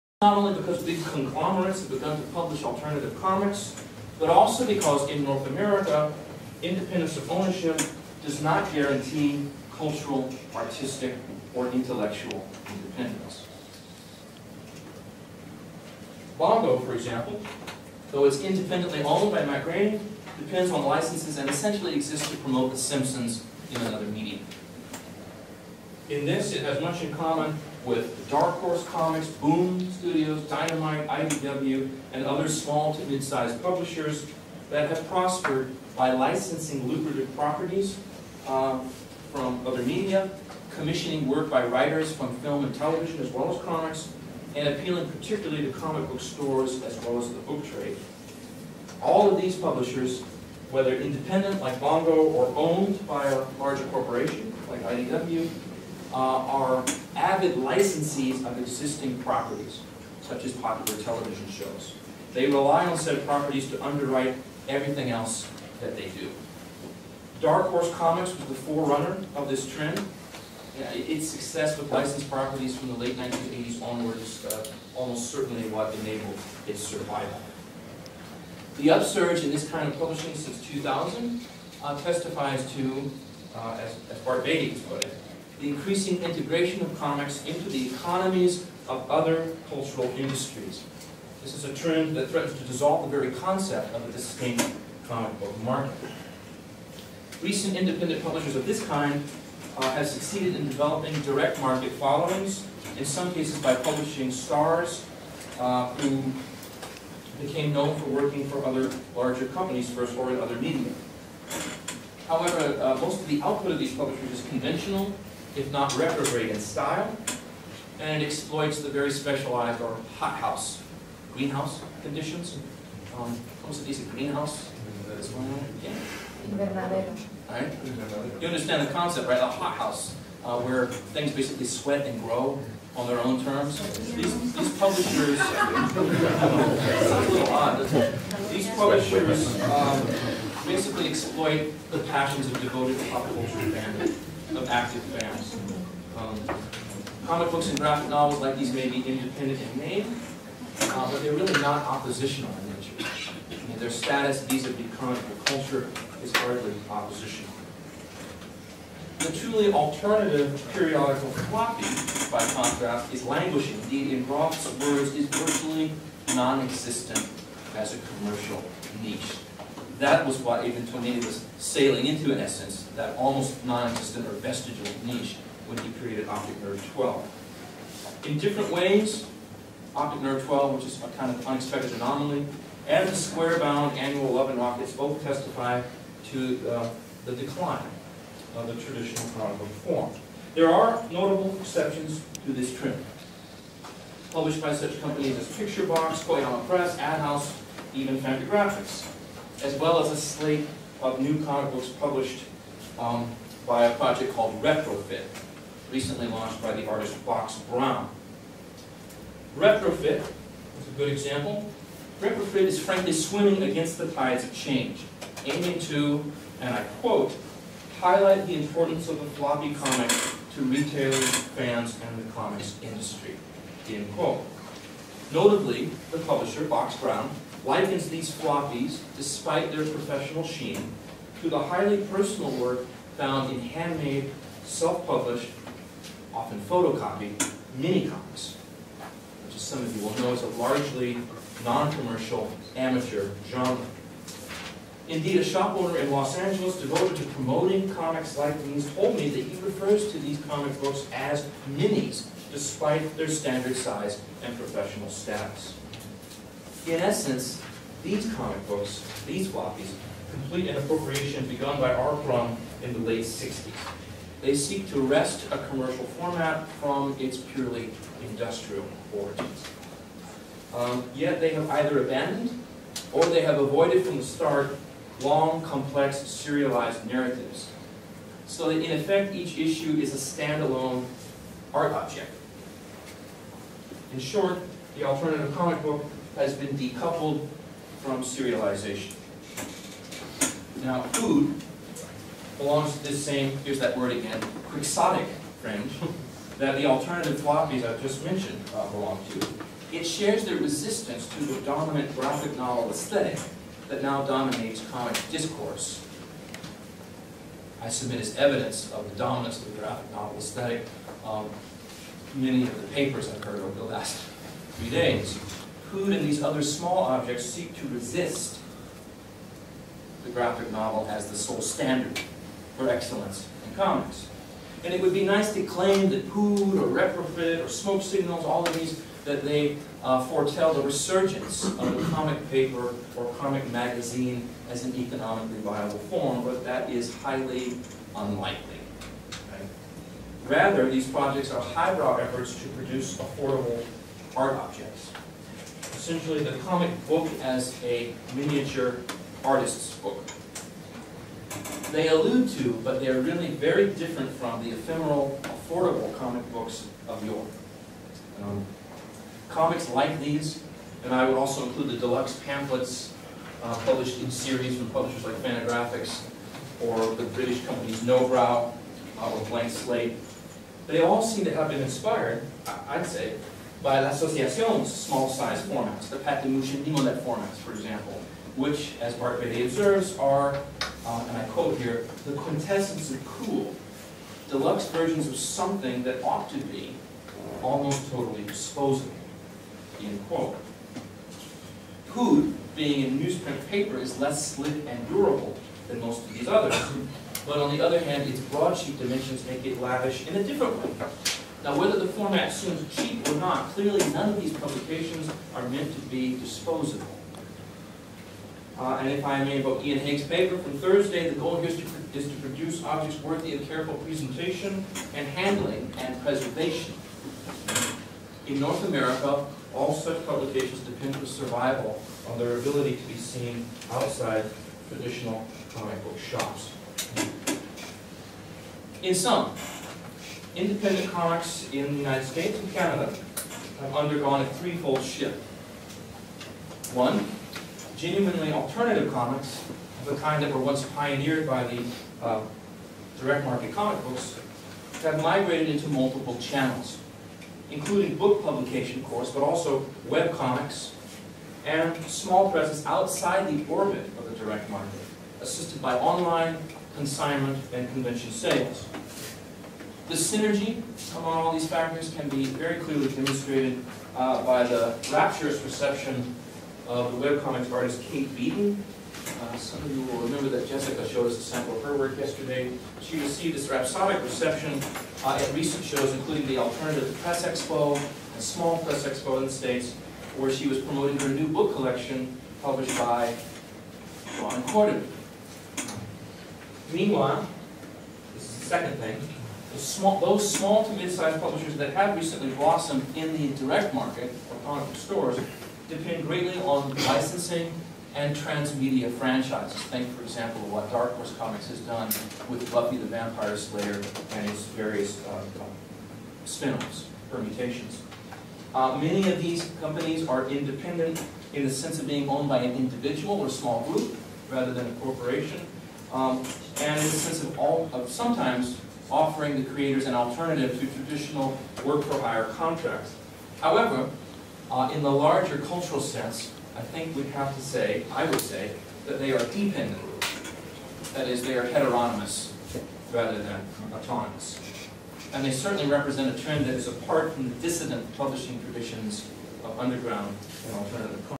Charles Hatfield - 2011-11-12 (1/4)
Part of the lecture by Charles Hatfield at the I International Congress of Comics and Grahic Novels in Alcalá de Henares (Spain).